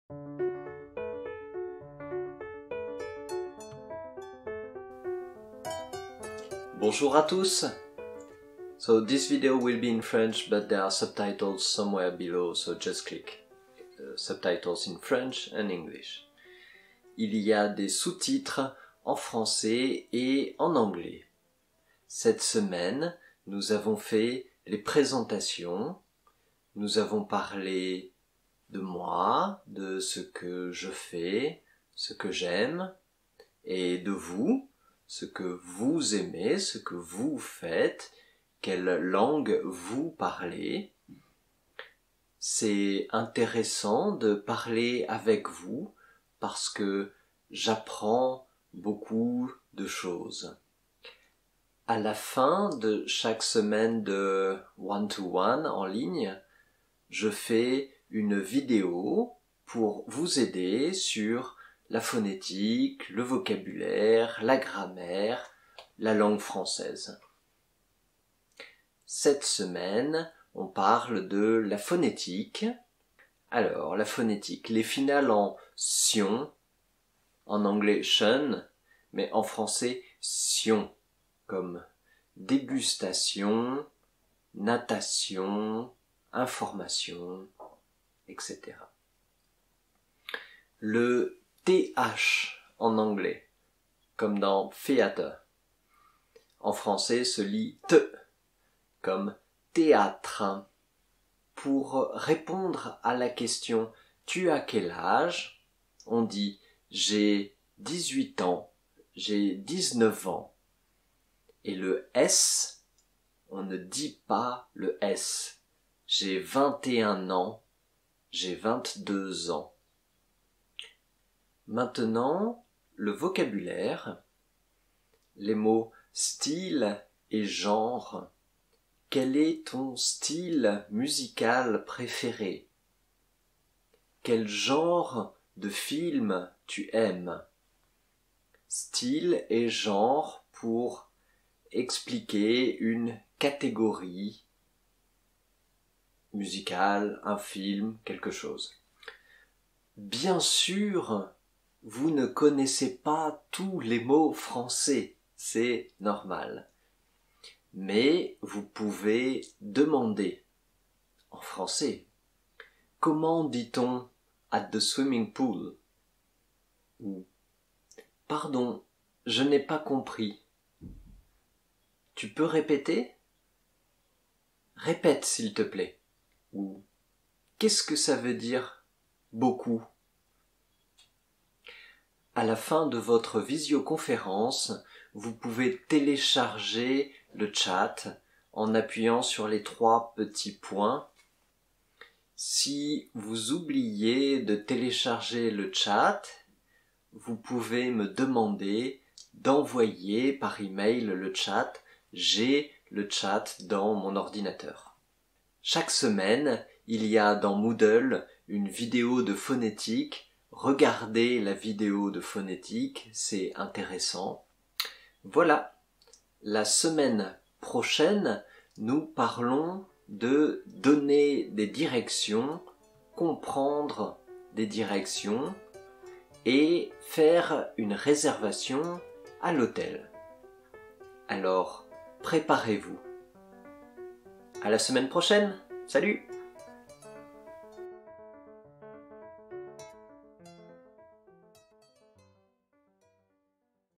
retour semaine 7: phonétique et vocabulaire
Here you'll get feedbacks about our discussions from week 7 and informations about next week.
Video quality can be improved by hitting the settings button.
Subtitles are in English only.